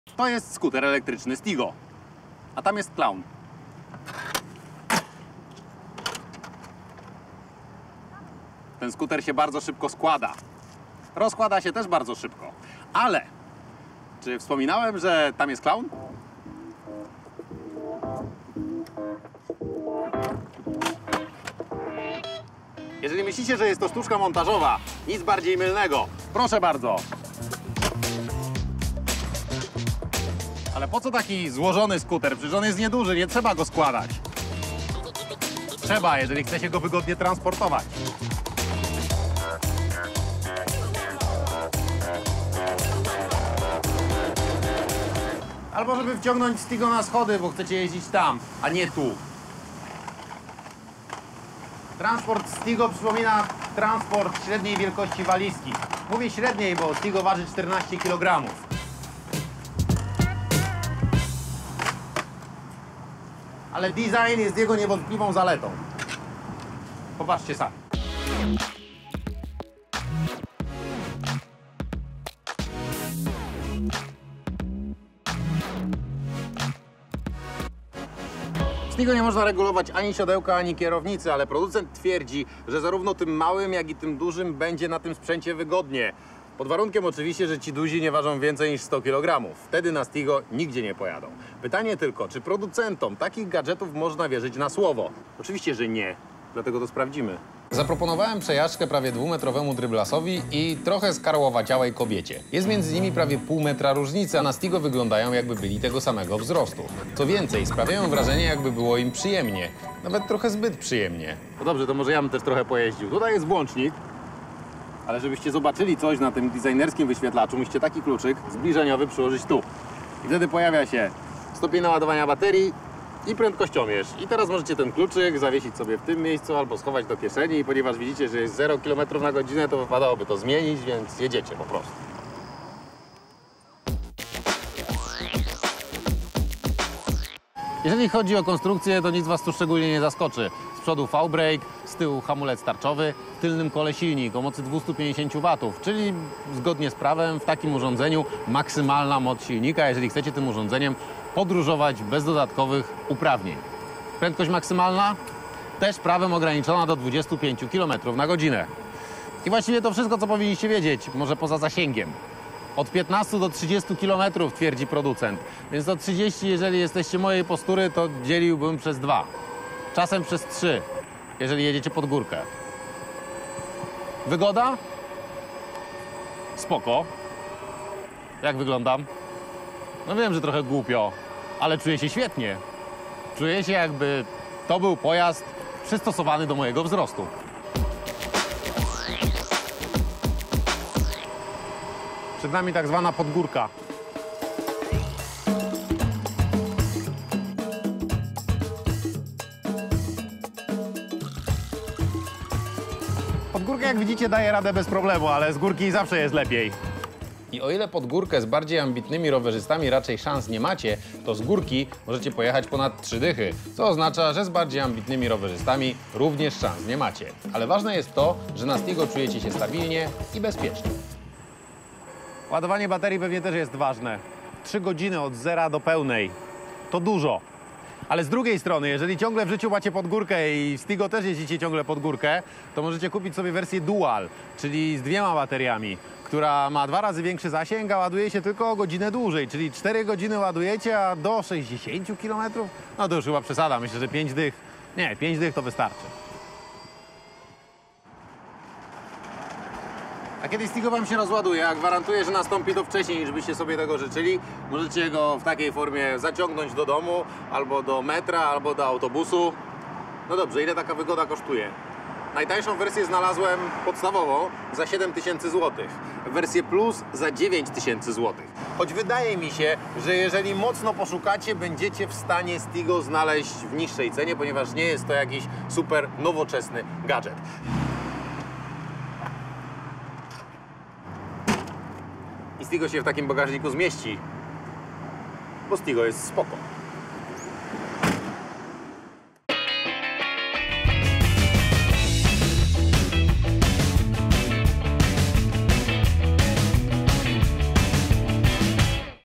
Ten mały skuter jest zadziwiająco wygodny [Nowy gadżet]
Kuba Klawiter przetestuje bidon z aplikacją, pojeździ czymś elektrycznym i wgra aplikację, która pokazuje... ile razy sprawdziłeś telefon. Przekonacie się też, jak działa świecąca książko-lampka, łapka do stabilizacji telefonu oraz składany kask.